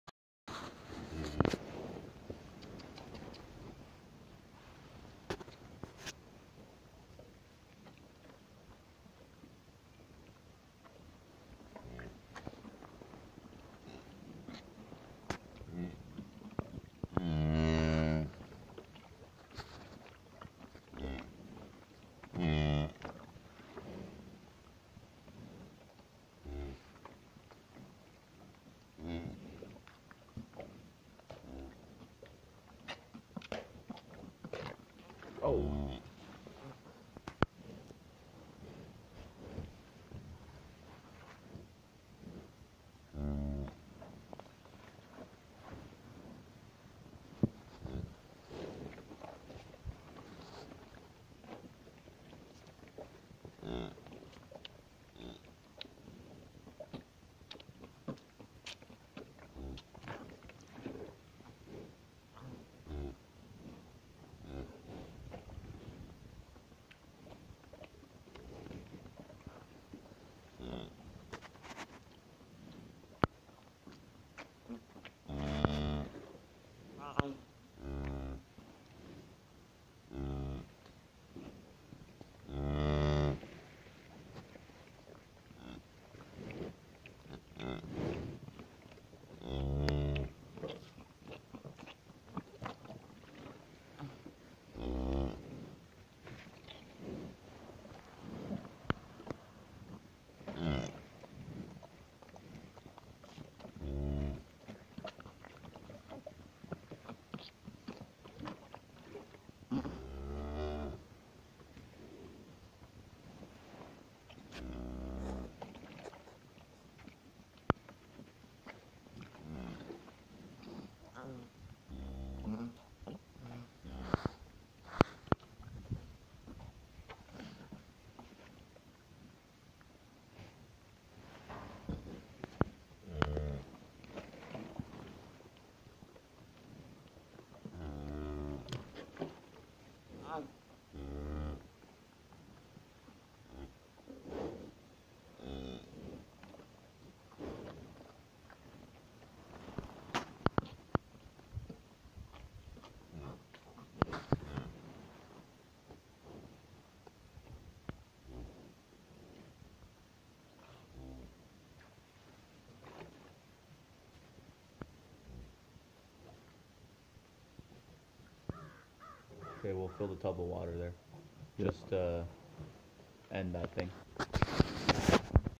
First calf of 2018
I'm giving the little guy a bottle to supplement his mother's milk so he gets more strength to drink off her on his own.